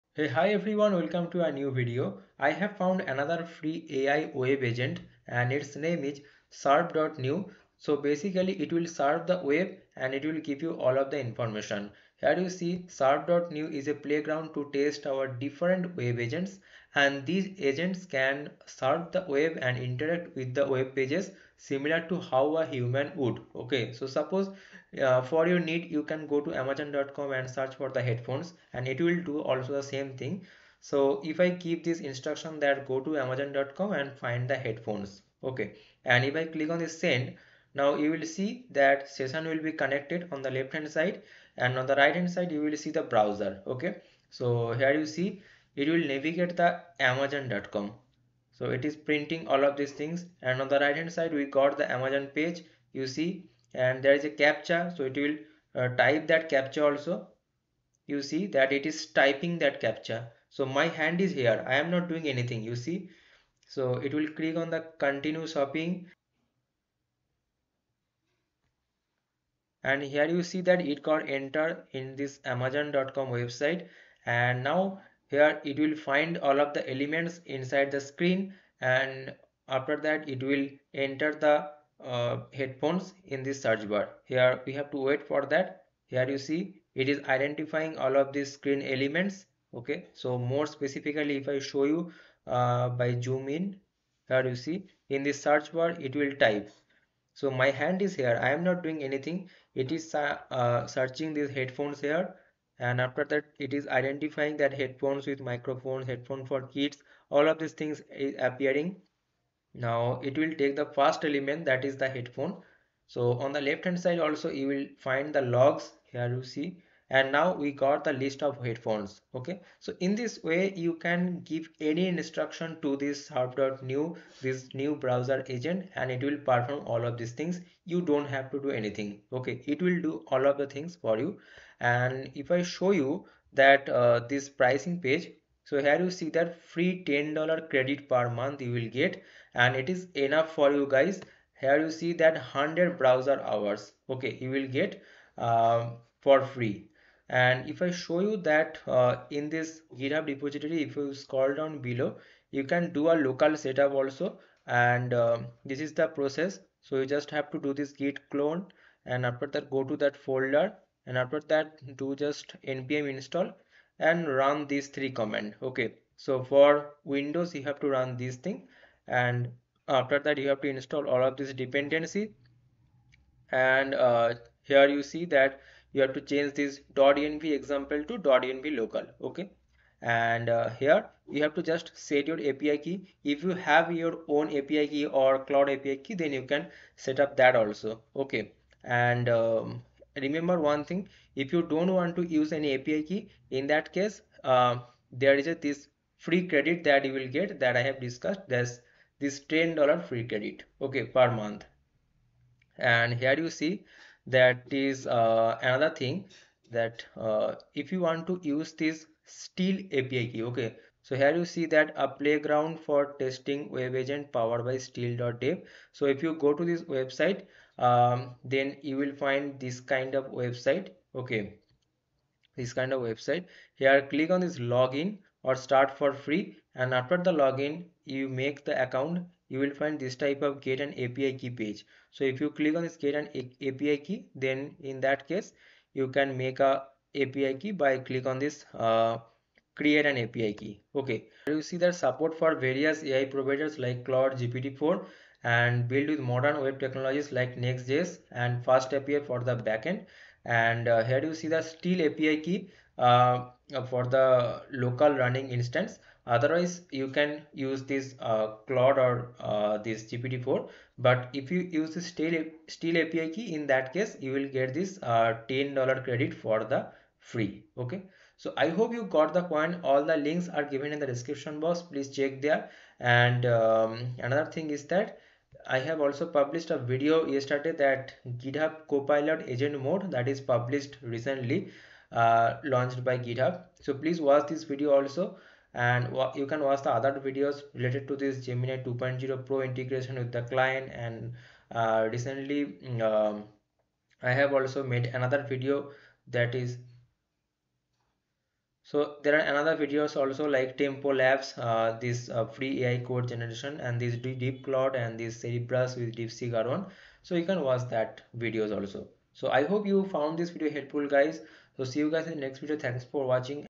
Surf.new: FREE AI Agent CAN CONTROL BROWSERS & DO ANYTHING! (Opensource)
Suft.new is a powerful open-source platform for deploying AI web agents to automate tasks like web scraping, form filling, captcha solving, and data collection—all for FREE!

🔗 **Resources Mentioned in the Video:**